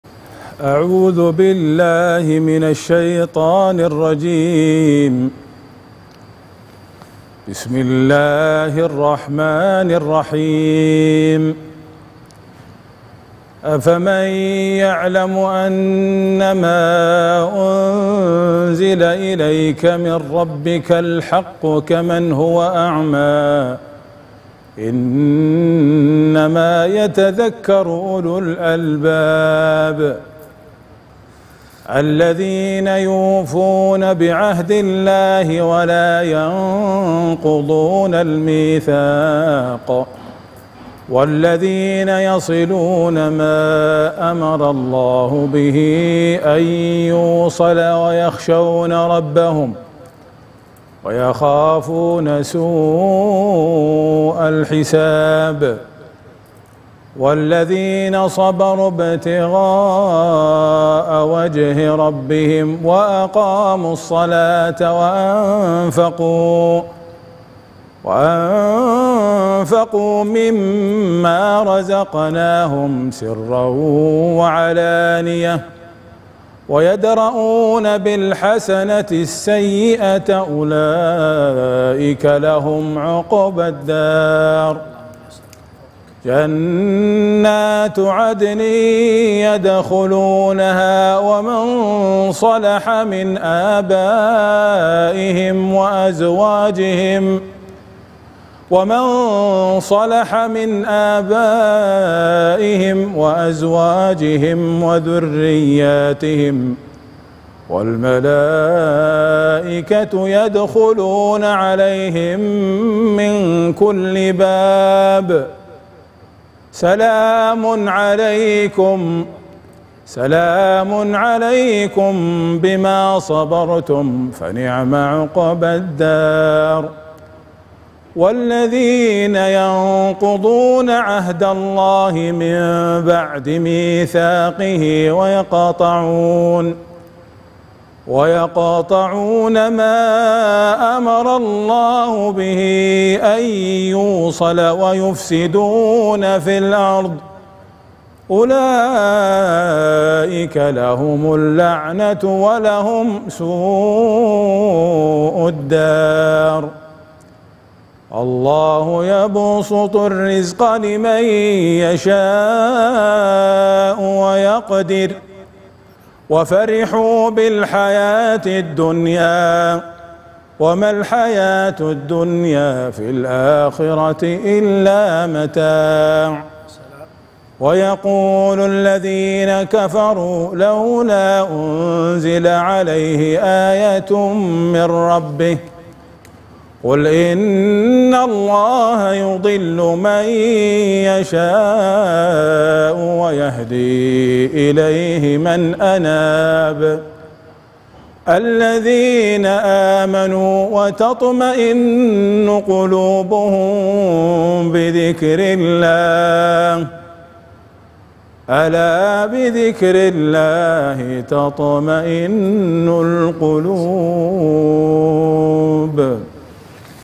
﴿... ألا بذكر الله تطمئن القلوب﴾ | تلاوة نجدية لآيات من سورة الرعد | القارئ : عبدالحميد النويشر
يوميًا في رمضان 1445 هـ 🌙 10:00 م

ويعاد:
على
يوميًا 2:00 م

وعلى
يوميًا 7:40 ص - 12:30 م

13 قناة آمنة لأسرتك!